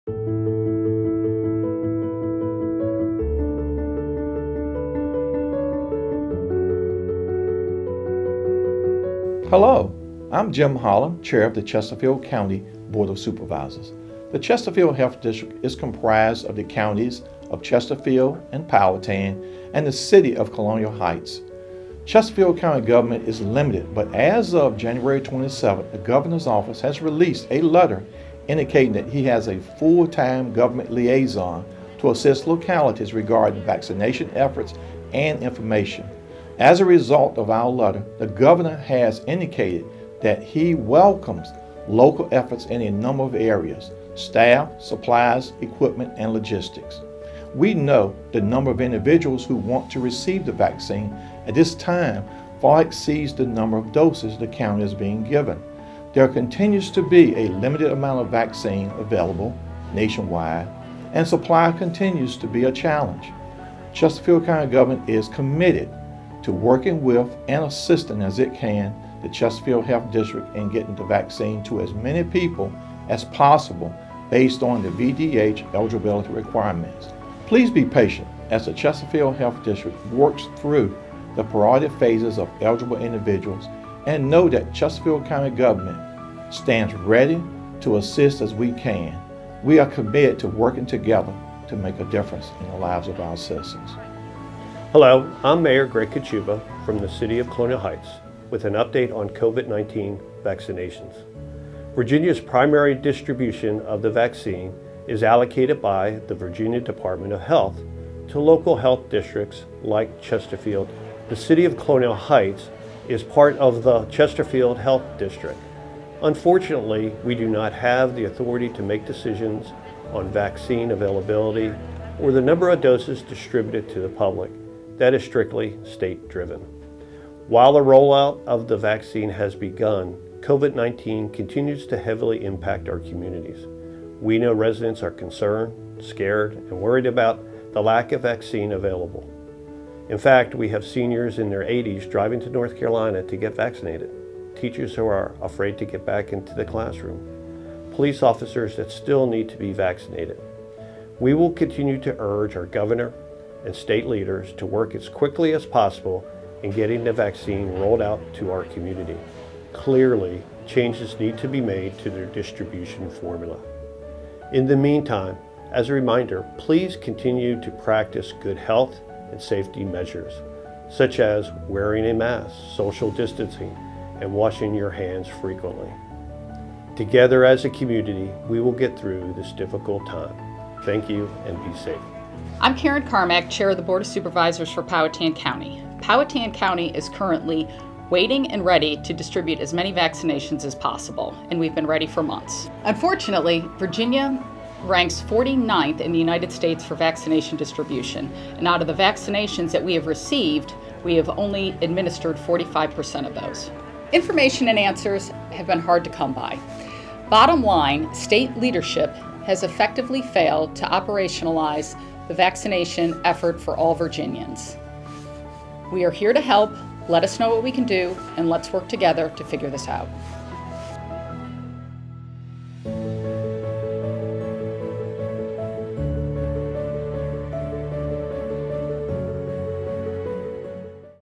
A Message From Elected Officials of the Chesterfield Health District
A message about the COVID-19 vaccine from Chesterfield County Board of Supervisors Chair Jim Holland, Colonial Heights Mayor Greg Kochuba and Powhatan County Board of Supervisors Chair Karin Carmack.